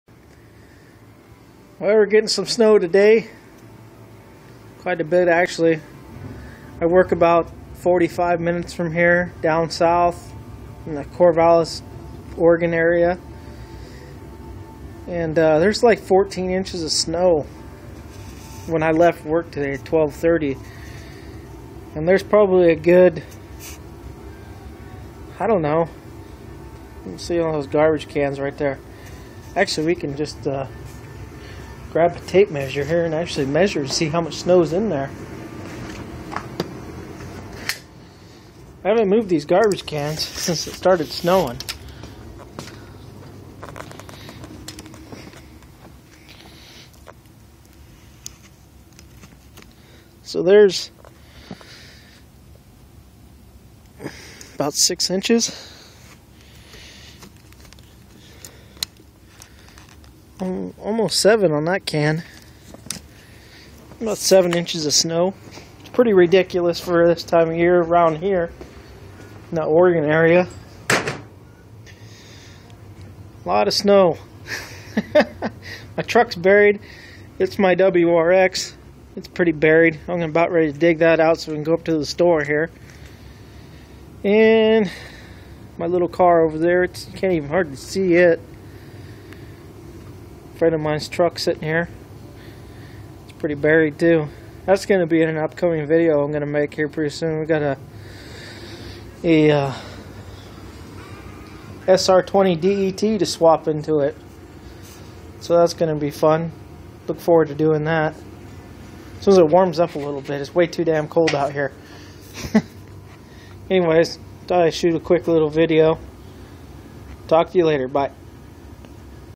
Oregon snow.
Good snow storm sweeping through the willamette valley this year.